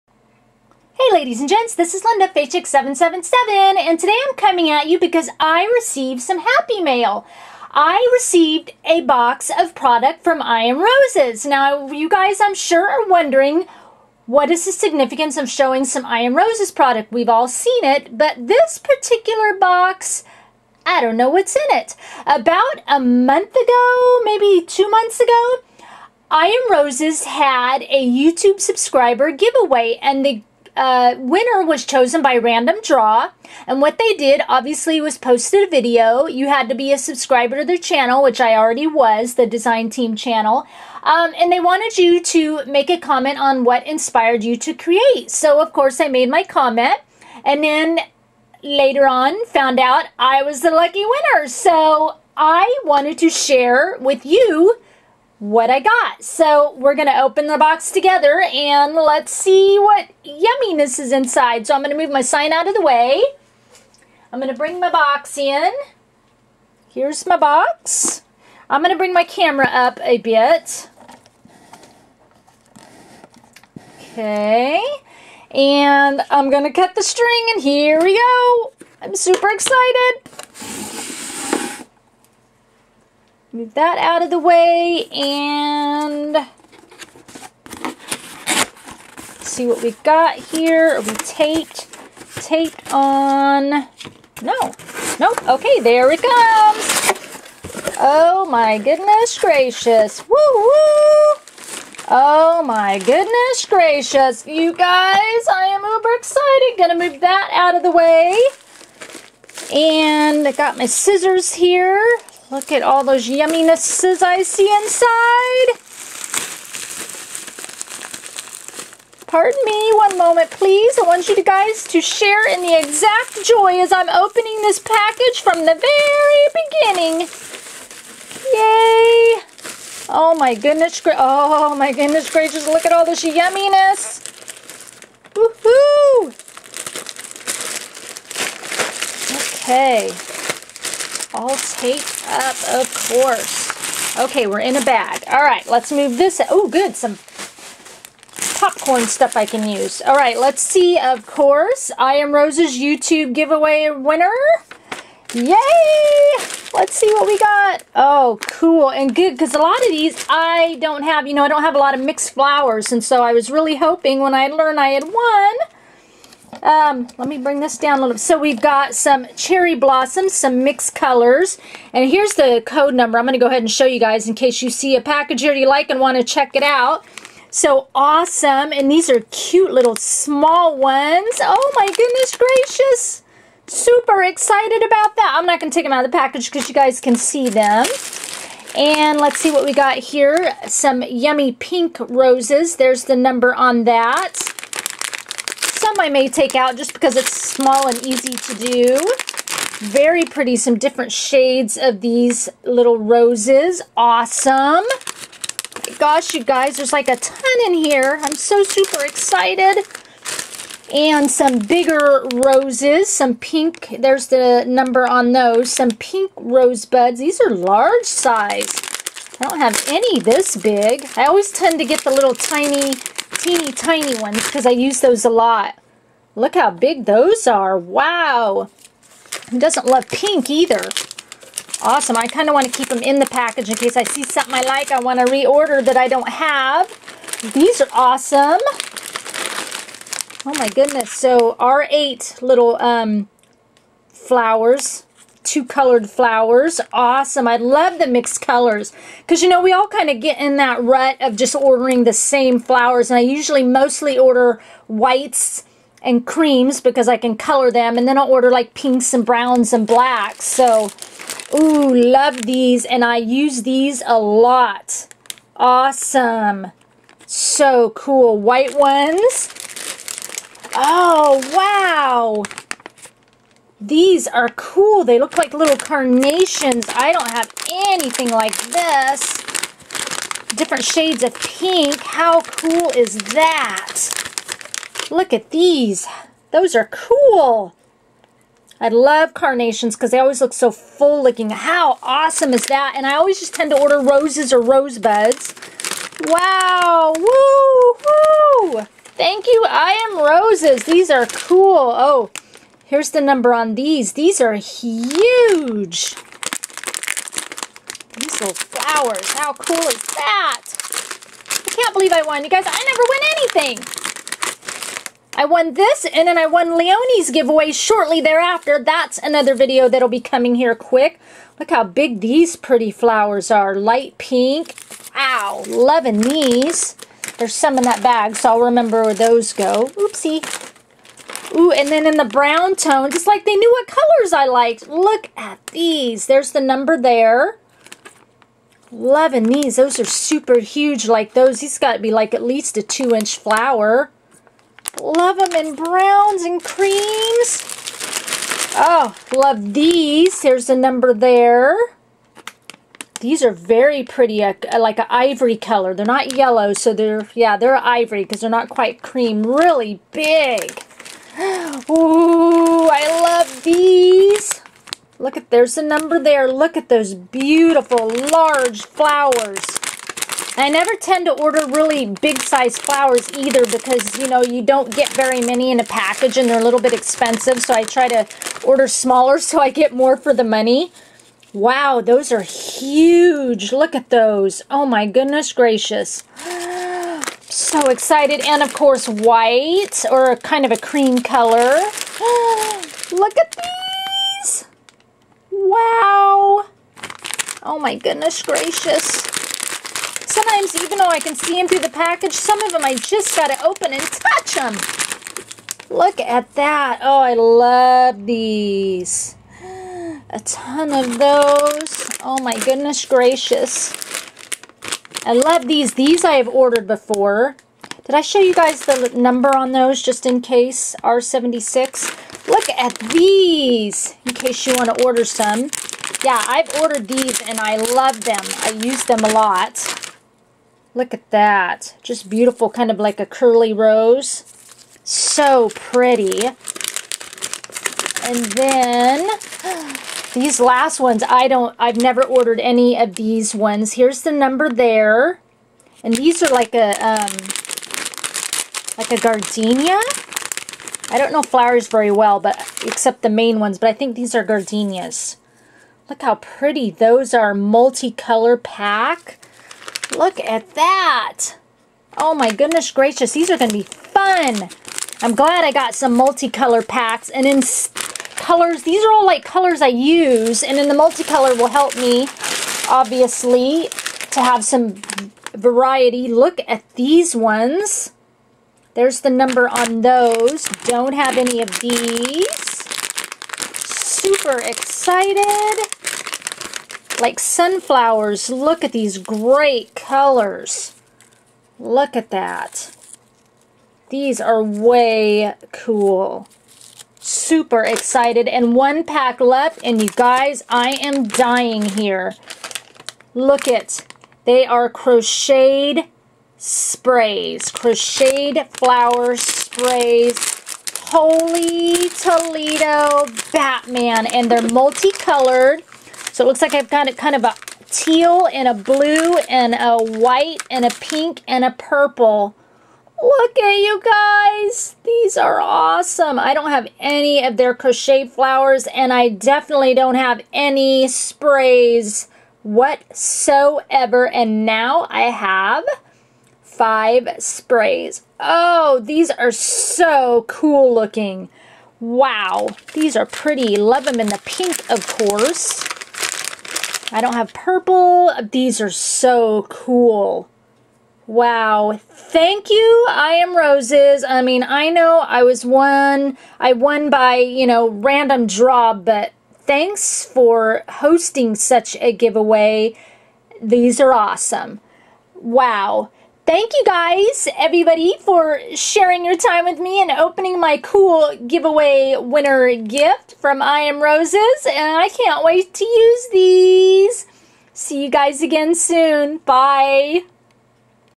Yumminess from IAR's YT subscriber giveaway contest!
This is the fabulous box of goodies that I received when I was chosen (by random draw) as the winner of the I Am Roses YT subscriber giveaway contest!  Such beautiful product--as usual! I am in love with the crochet flowers!  I will be definately ordering more of those!
xoxoxoxo (btw--which is the hugsies and which is the kissies? LOL)